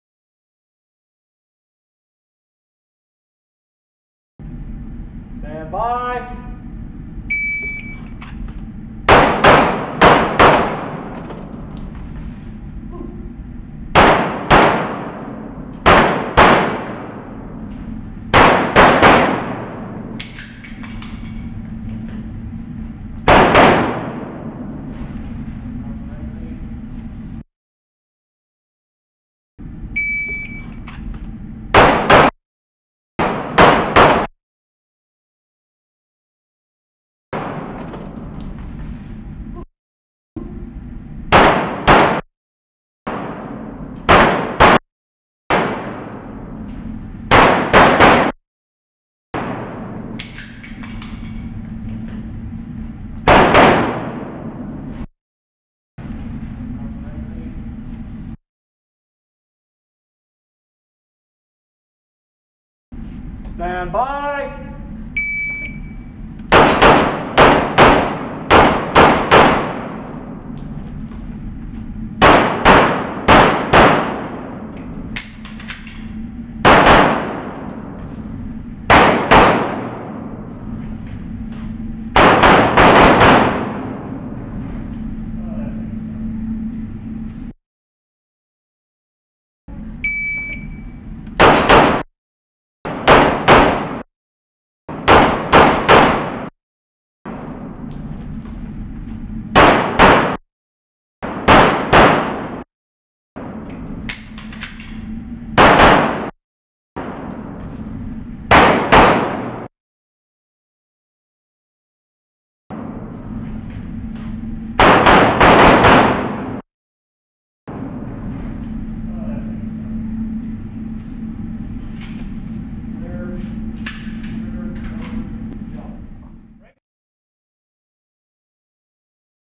Jan 2014 IDPA Match at Thunder Alley
Here's some video of the two stages of the January 2014 IDPA match at Thunder Alley in Lincoln, NE.  Difference between first and second stages was that the second stage had three more targets spaced out between the arrays, necessitating reloading elsewhere.  (Plus the firearm started on the table instead of being in the holster.)  Shooting positions were the same, though.

My shooting was decently fast, but it would have helped if I had aimed more.  On each stage I pulled a mike about half an inch into hard cover.  [sigh]  Down 13 points total.